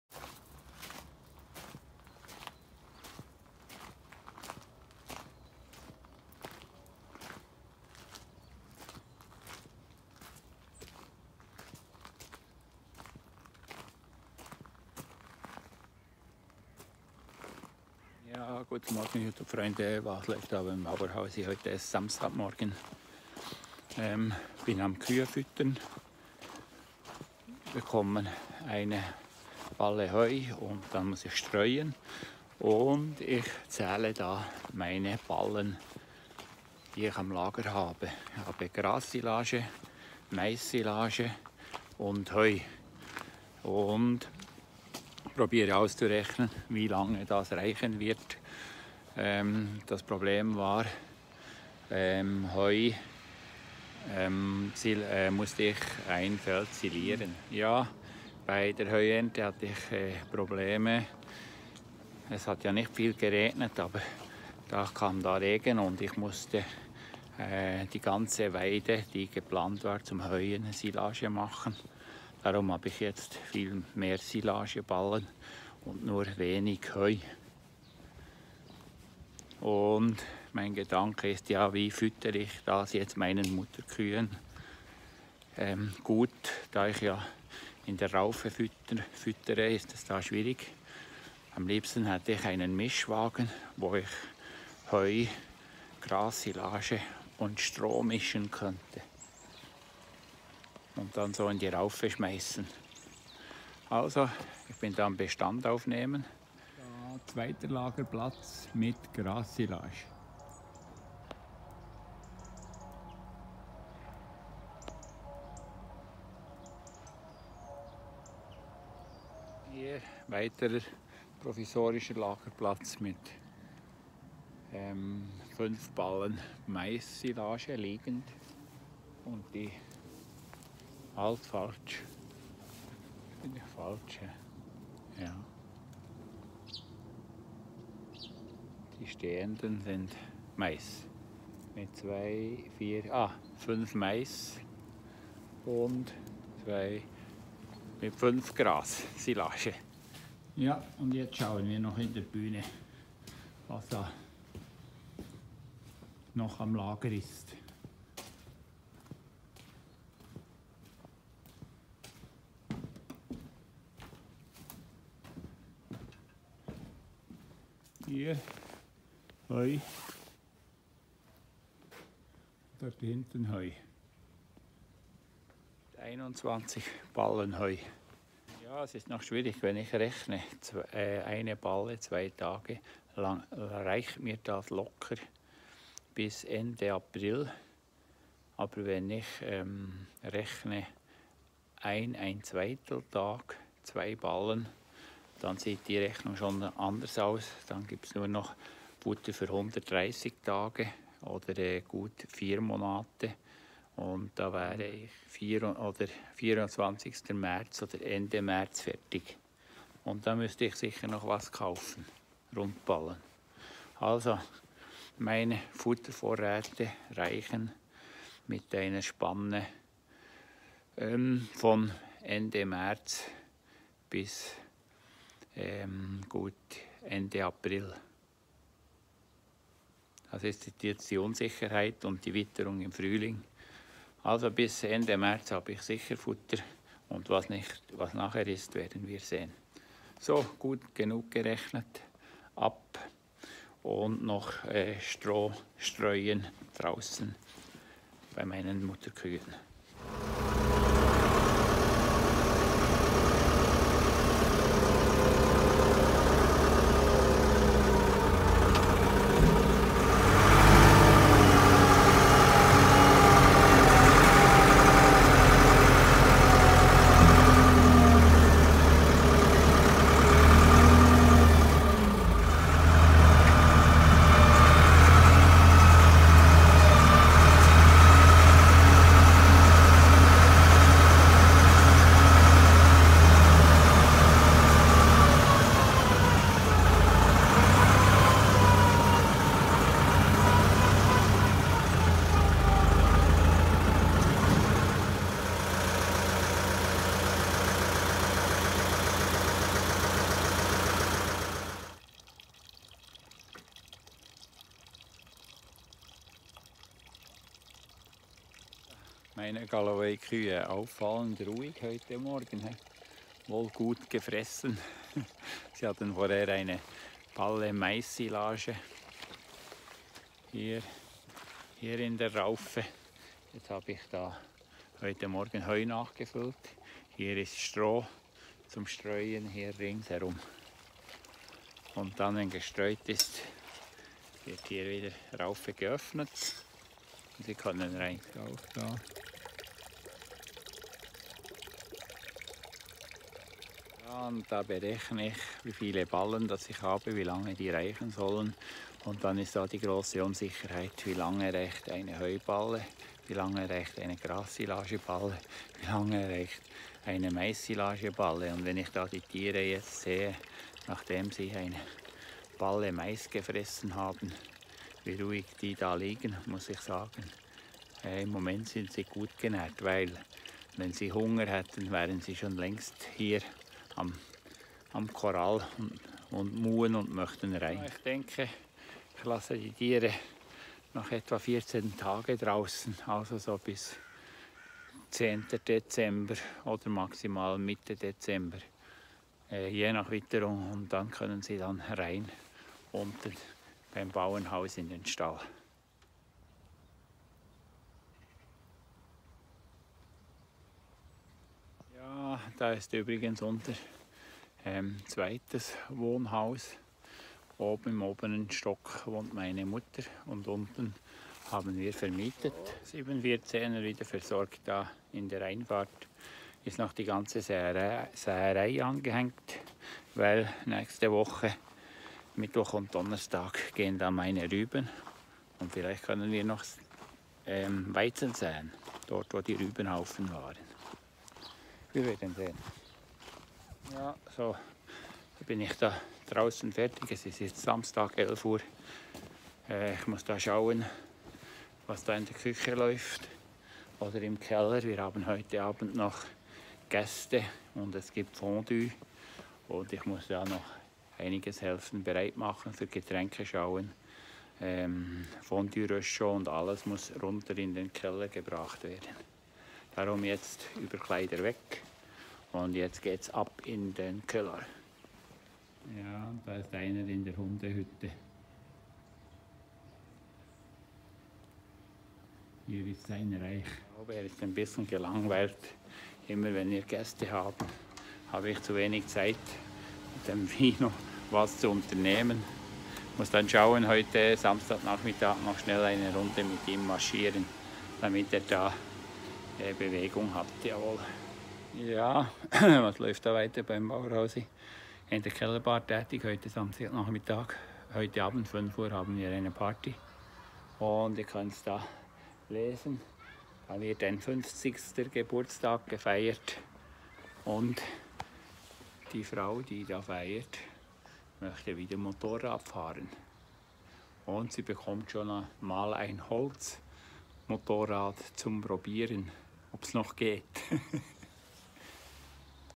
Der Bauer 👨‍🌾 Housi rechnet......reicht das Futter 🤔
Reichen meine Futtervorräte über den Winter